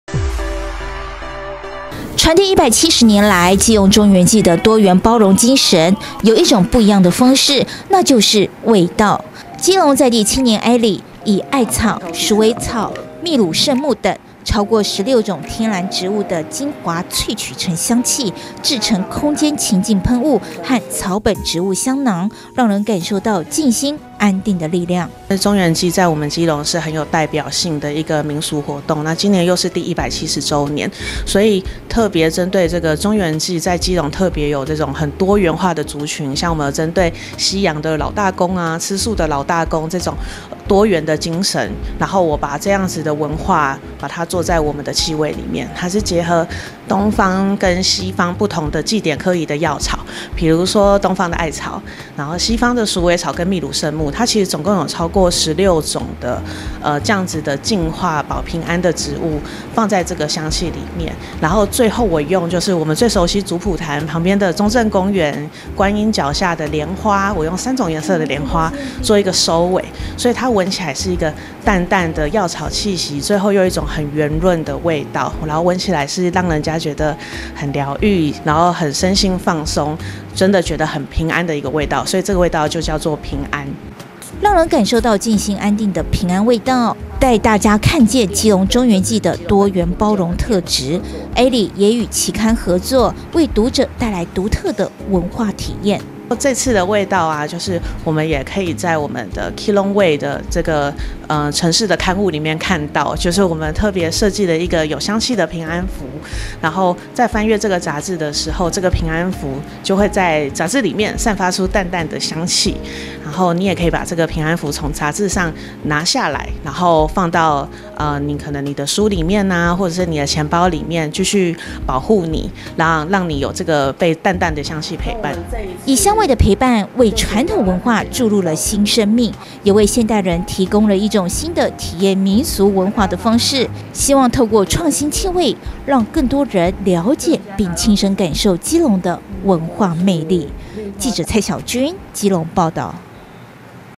基隆 香氛|創新保平安 中元祭香氣喚醒城市記憶
（中嘉新聞記者 蔡曉君/基隆採訪報導）基隆在地青年Elly攜手期刊合作，以鷄籠中元祭為靈感，推出「平安之味」香氣系列。融合了傳統民俗與現代生活，將東西方淨化藥草的精華萃取成香氣，傳遞170年來鷄籠中元祭的多元包容精神。

以艾草、鼠尾草、秘魯聖木等超過16種天然植物的精華萃取成香氣，製成空間情境噴霧和草本植物香囊，讓人感受到靜心安定的力量。

讓人感受到靜心安定的平安味道，帶大家感受鷄籠中元祭的多元包容特質，Elly也與期刊合作，為讀者帶來獨特的文化體驗。

以香味的陪伴，為傳統文化注入新生命，也為現代人提供了一種體驗民俗文化的方式，希望透過創新氣味，讓更多人了解並親身感受基隆的文化魅力。